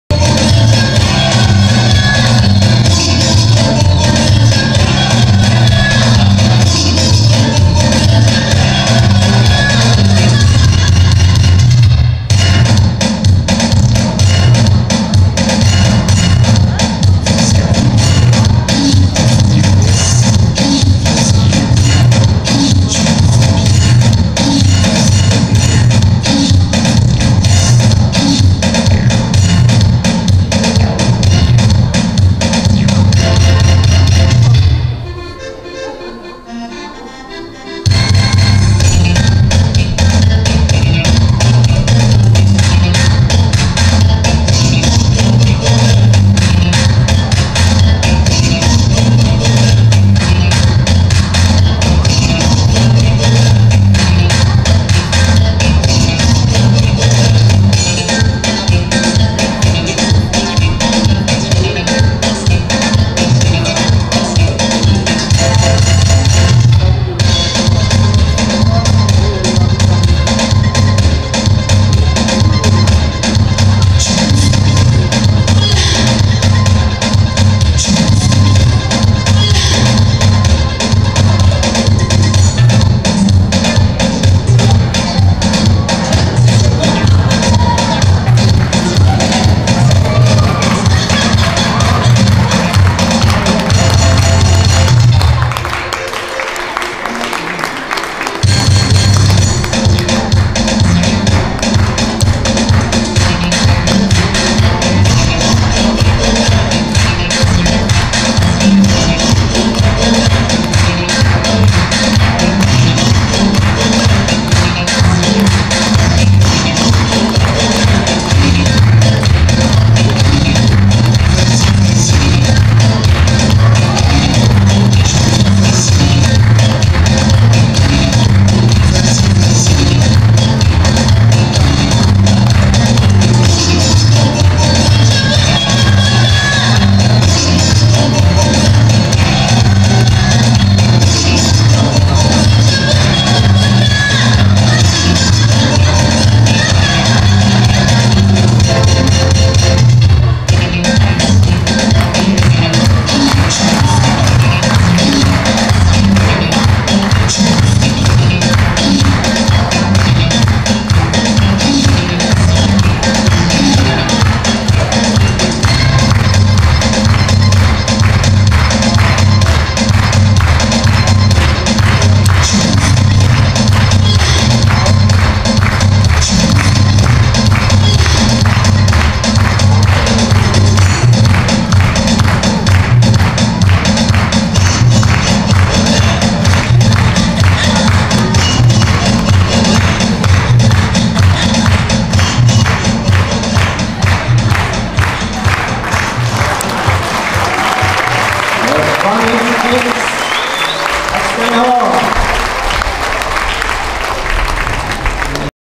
Sveriges nationaldag, den 06 juni 2009 , Sundsvall
Sveriges nationaldag, den 06 juni 2009 , Sundsvall, The Fanny Chicks  (8 tjejer från Siera Leone)